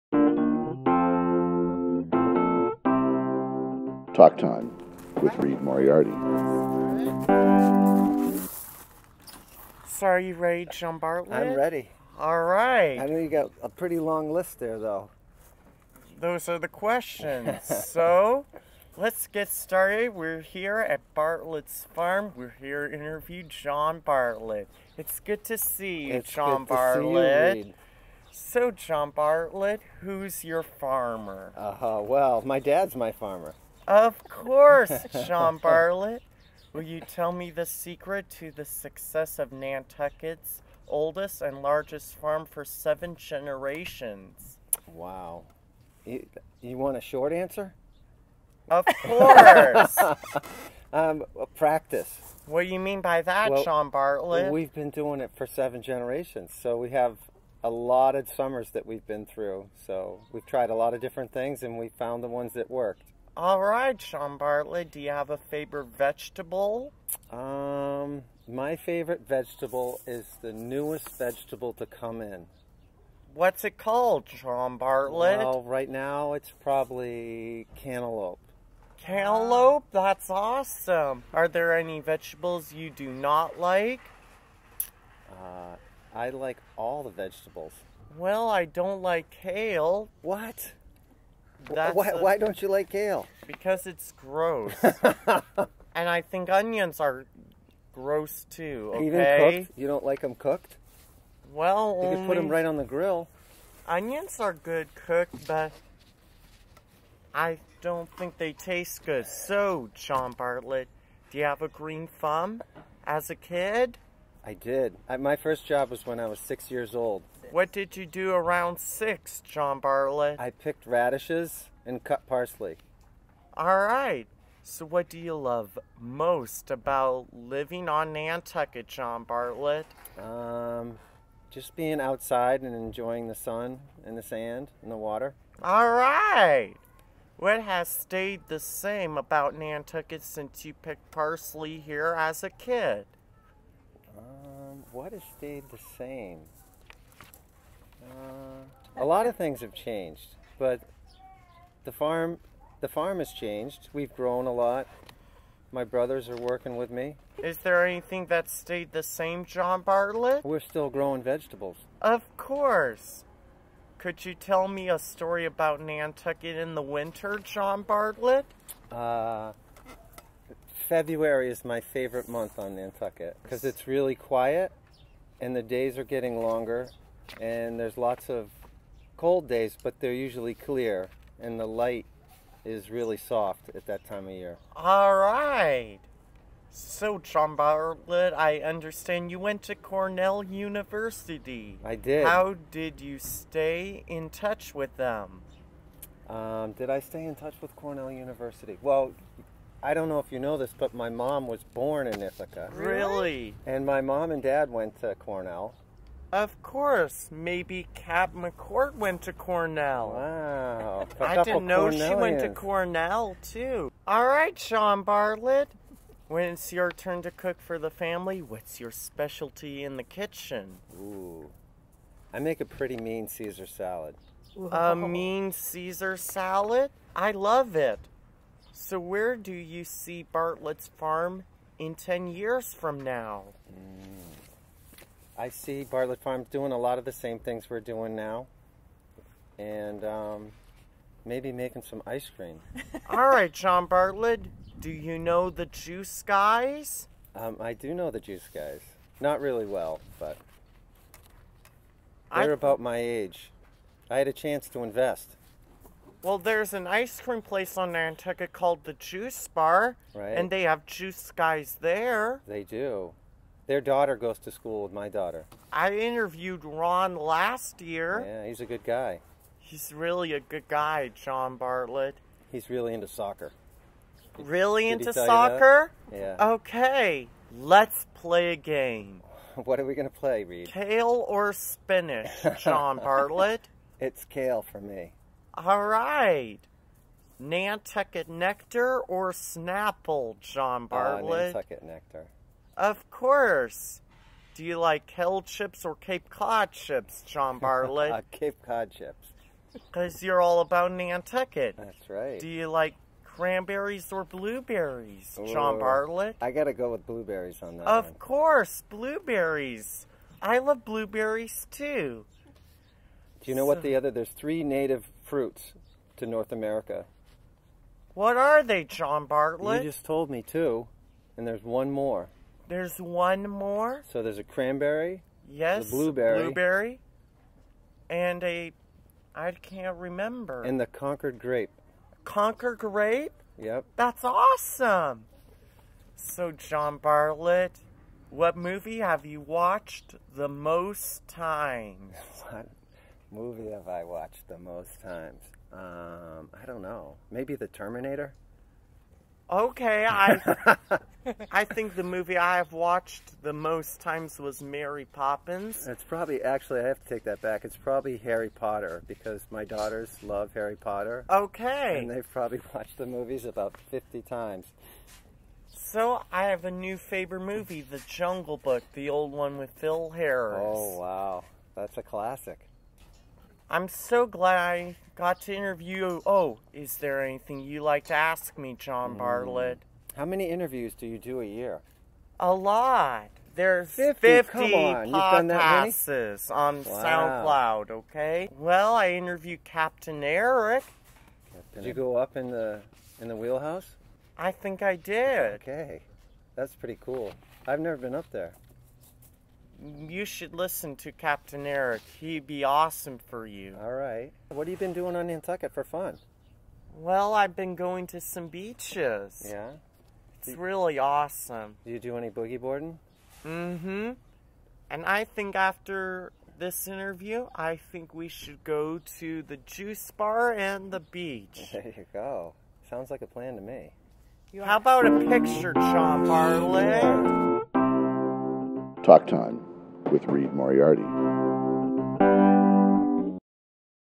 Talk Time with Reid Moriarty: John Bartlett, Bartlett's Farm, Nantucket
Bartlett's Farm is a beloved landmark and seventh generation farm, market, and greenhouse on the the faraway isle of Nantucket. Reid sat down with John Bartlett, who is currently running the 125-acre operation as President and farmer.

"Talk Time with Reid Moriarty" is a series of 7 minute interviews with people Reid finds interesting, and you might too!